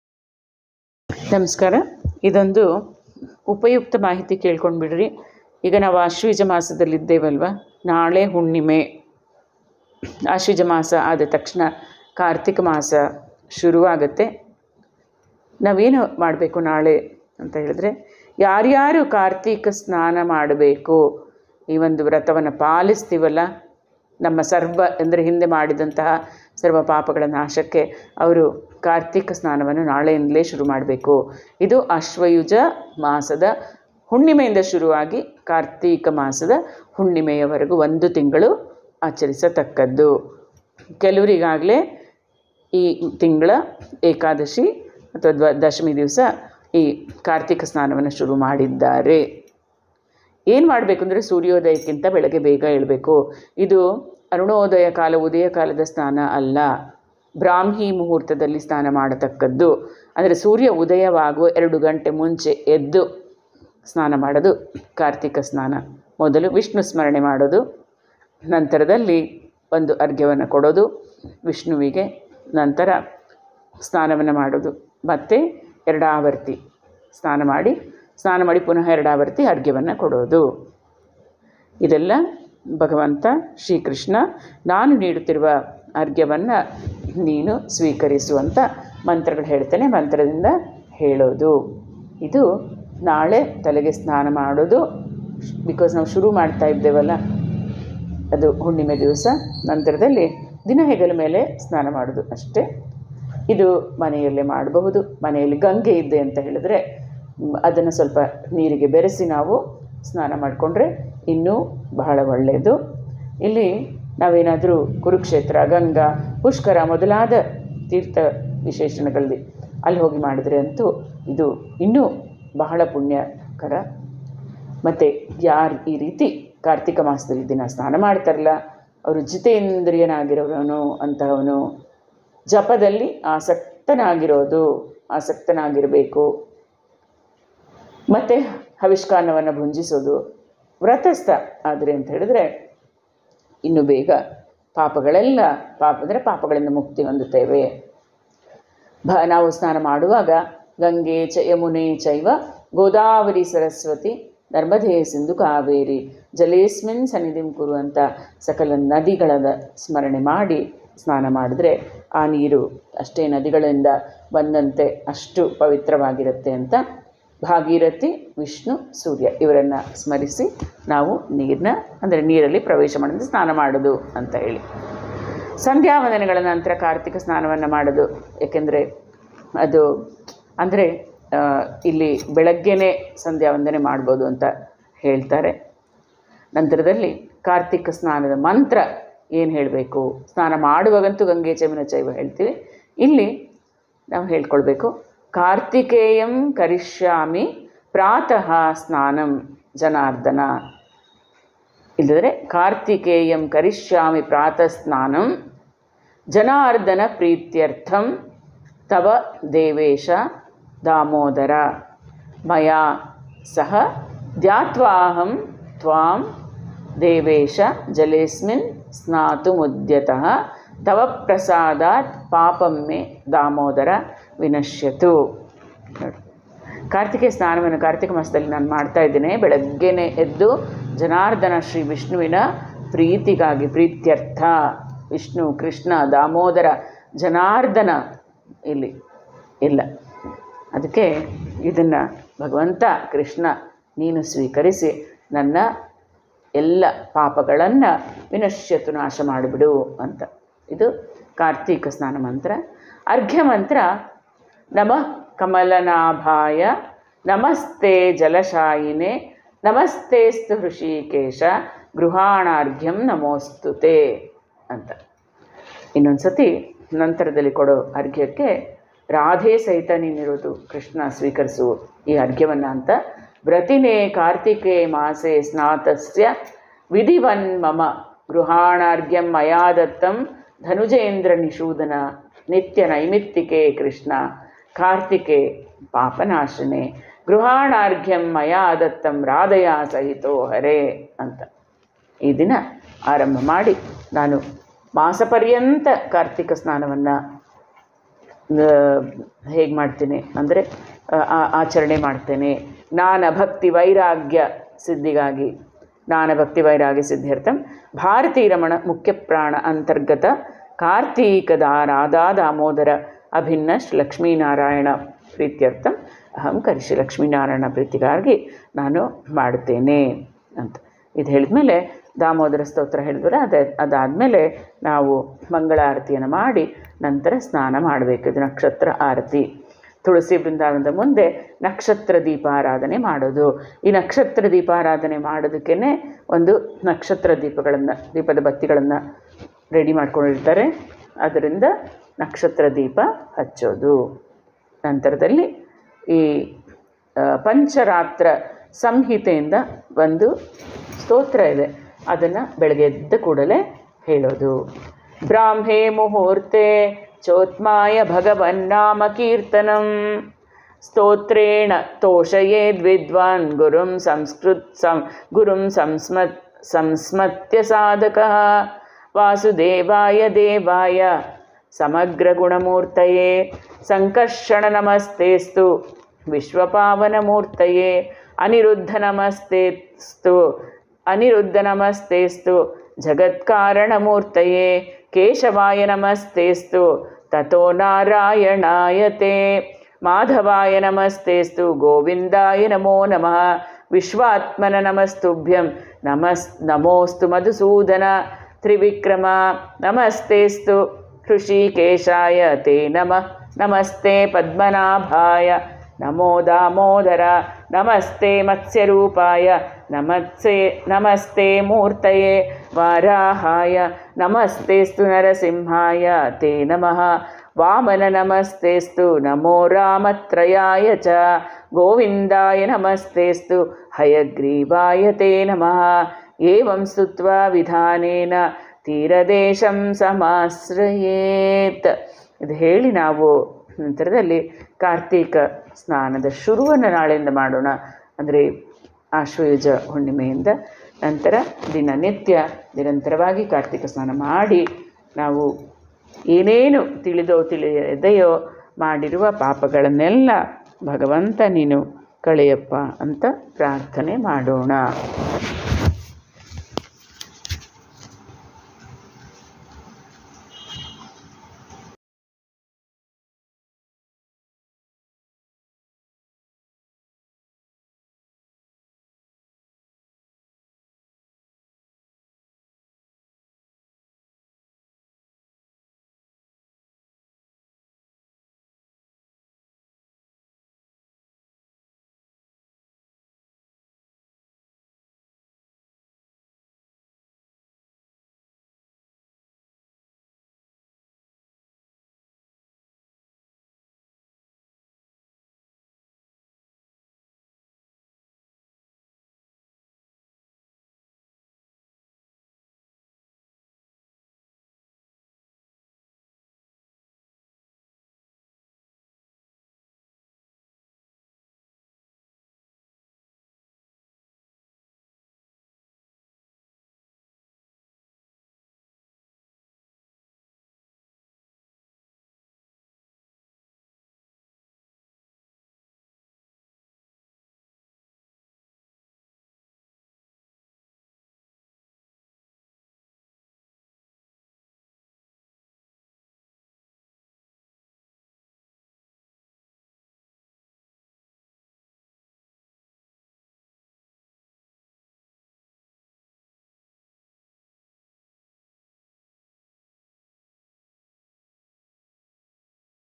ಇಂದಿನಿಂದಲೇ ಬೇಗ ಎದ್ದು ಕಾರ್ತೀಕ ಸ್ನಾನ ಶುರುಮಾಡಿ| ಫಲಗಳು ಅನೇಕ Kartika Snanam
ಇಲ್ಲಿವೆ ಹೇಳಬೇಕಾದ ಸ್ತೋತ್ರ,ಮಂತ್ರಗಳು
ನಾಳೆಯಿಂದಲೇ  ಬೇಗ ಎದ್ದು ಕಾರ್ತೀಕ ಸ್ನಾನ ಶುರುಮಾಡಿ| ಫಲಗಳು ಅನೇಕ Kartika Snanam

Krupe: FB HINDU SAMSKRUTI VAIBHAVA

ಕಾರ್ತಿಕ ಸ್ನಾನ ಮಂತ್ರ: ಕಾರ್ತಿಕೇಯಂ ಕರಿಷ್ಯಾಮಿ ಪ್ರಾತಃ ಸ್ನಾನಂ ಜನಾರ್ದನ ಪ್ರೀತ್ಯರ್ಥಂ ತವ ದೇವೇಶ ದಾಮೋದರ ಮಯಾ ಸಹ ಧ್ಯಾತ್ವಾಹಂ ತ್ವಾಂ ದೇವೇಶ ಜಲೇಸ್ಮಿನ್ ಸ್ನಾತು ಮುದ್ಯತಃ ತವ ಪ್ರಸಾದಾತ್ ಪಾಪಂ ಮೇ  ದಾಮೋದರ ವಿನಶ್ಯತು.

ಆರ್ಘ್ಯ ಮಂತ್ರ:
ನಮಃ ಕಮಲನಾಭಾಯ ನಮಸ್ತೆ ಜಲಾಶಾಯಿನೇ ನಮಸ್ತೆಸ್ತು ಋಷಿಕೇಷ ಗೃಹಾಣರ್ಘಂ ನಮೋಸ್ತುತೆ!
ವ್ರತಿನೇ ಕಾರ್ತಿಕ ಮಾಸೇ ಸ್ನಾತಸ್ಯ ವಿಧಿವನ್ಮಮ  ಗ್ರಹಣಾರ್ಘಂ  ಮಯಾದತ್ತಂ ಧನುಜೇಂದ್ರ ನಿಷೂಧನ !! ನಿತ್ಯ ನೈಮಿತ್ತಿಕೇ  ಕೃಷ್ಣ ಕಾರ್ತಿಕೇ ಪಾಪನಾಶನೇ ಗೃಹಾಣರ್ಘ್ಯಂ ಮಯಾ ದತ್ತಂ ರಾಧಯಾ ಸಹಿತೋ ಹರೇ!!*

ಪಂಚರಾತ್ರ ಸಂಹಿತೆಯಿಂದ  ಒಂದು ಸ್ತೋತ್ರ ಬೆಳಗ್ಗೆ ಎದ್ದ ಕೂಡಲೇ ಹೇಳಲು

ಬ್ರಾಹ್ಮೇ ಮುಹೂರ್ತೇ ಚೋತ್ಥಾಯ ಭಗವನ್ನಾಮಕೀರ್ತನಂ! ಸ್ತೋತ್ರೇಣ ತೋಷಯೇದ್ ವಿದ್ವಾನ್ ಗುರುಮ್ ಸಂಸ್ಮತ್ಯ ಸಾಧಕಃ ! ವಾಸುದೇವಾಯ ದೇವಾಯ ಸಮಗ್ರ ಗುಣ ಮೂರ್ತಯೇ ಸಂಕರ್ಷಣ ನಮಸ್ತೆಸ್ತು ವಿಶ್ವ ಪಾವನ ಮೂರ್ತಯೇ ಅನಿರುದ್ಧ ನಮಸ್ತೆಸ್ತು ಜಗತ್ಕಾರಣ ಮೂರ್ತಯೇ ಕೇಶವಾಯ ನಮಸ್ತೆಸ್ತು ತತೋ ನಾರಾಯಣಾಯ ತೇ ಮಾಧವಾಯ ನಮಸ್ತೆಸ್ತು ಗೋವಿಂದಾಯ ನಮೋ ನಮಃ ವಿಶ್ವಾತ್ಮನ ನಮಸ್ತುಭ್ಯಂ ನಮೋಸ್ತು ಮಧುಸೂದನ ತ್ರಿವಿಕ್ರಮ ನಮಸ್ತೆಸ್ತು ಹೃಷಿಕೇಶಾಯ ತೇ ನಮಃ ನಮಸ್ತೆ ಪದ್ಮನಾಭಯ್ಯ ನಮೋ ದಾಮೋದರ ನಮಸ್ತೆ ಮತ್ಸ ರೂಪಾಯ ನಮಸ್ತೆ ಮೂರ್ತಯೇ ವಾರಾಹಯ ನಮಸ್ತೆಸ್ತು ನಾರಸಿಂಹಾಯ ತೇ ನಮಃ ವಾಮನ ನಮಸ್ತೆಸ್ತು ನಮೋ ರಾಮ ತ್ರಯಾಯ ಚ ಗೋವಿಂದಾಯ ನಮಸ್ತೆಸ್ತು ಹಯಗ್ರೀವಾಯ ತೇ ನಮಃ ಏವಂಸ್ತುತ್ವಾ ವಿಧಾನೇನ ತೀರದೇಶಂ ಸಮಾಸ್ರಯೇತ್ !.

Kartikamasa
Puje
Purana
Snana
Arghya
Krishna
Shiva
Ganesha
Stotras
Kannada bhajans
Hunnime
Kojagari